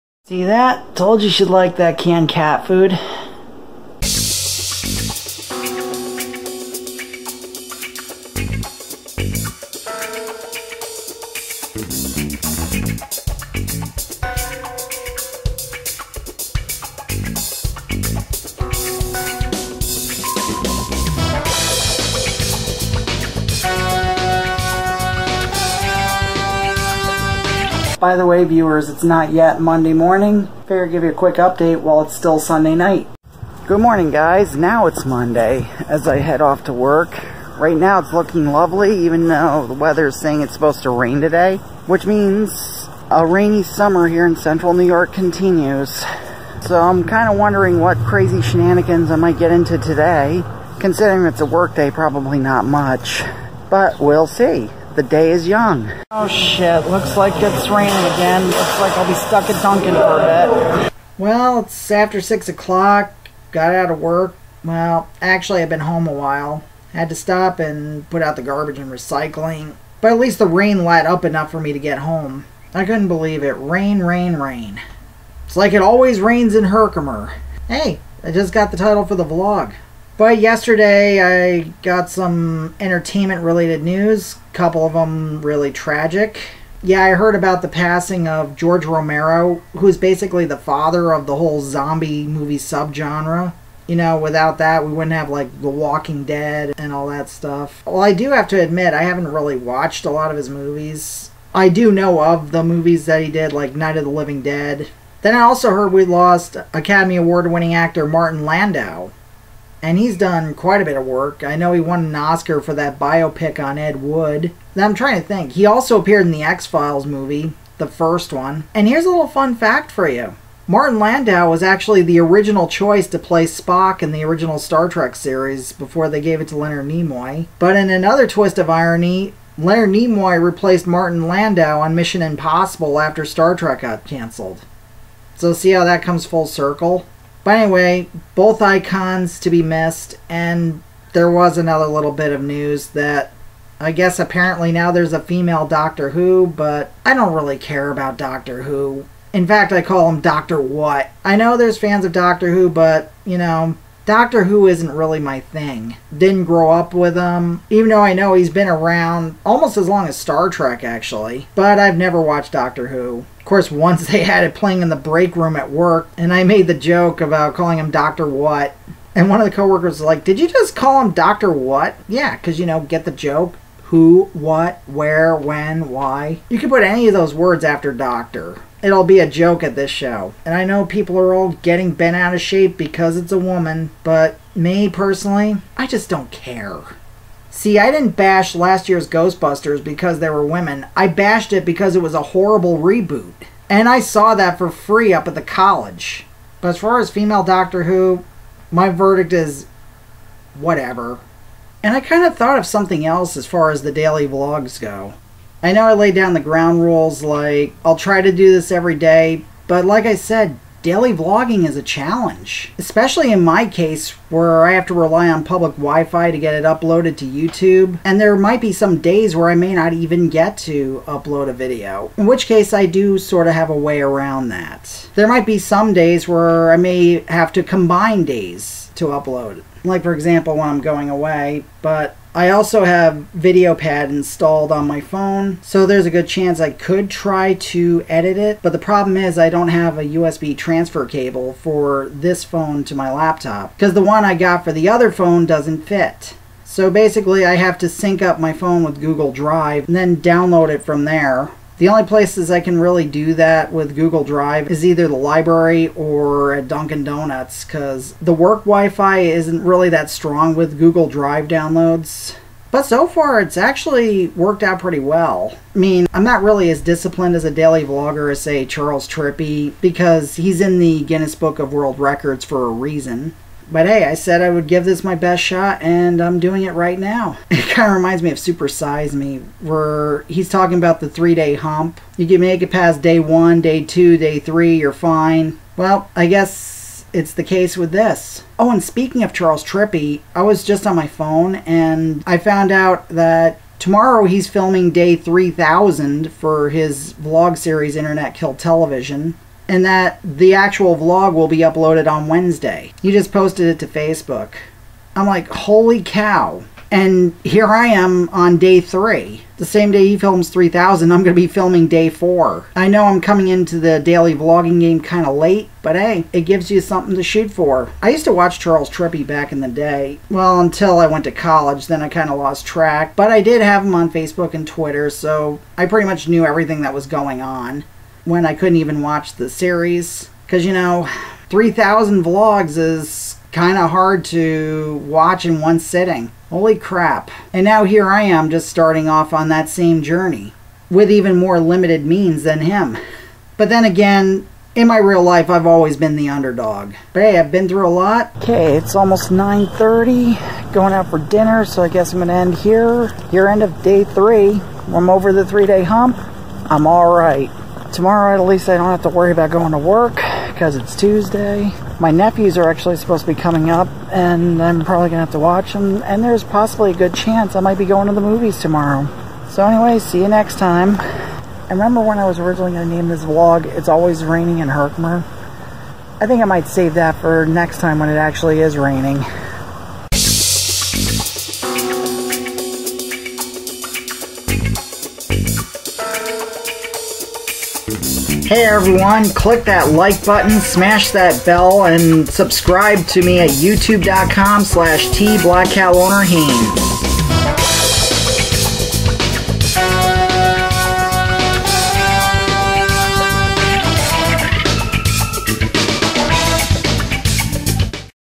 THE THREE-DAY HUMP (#3- 7/17/17)
Day 3 of my daily vlog finds me talking about everything from Doctor Who to another milestone in Charles Trippy's daily vlog career. And there's also a night vision shot toward the end too.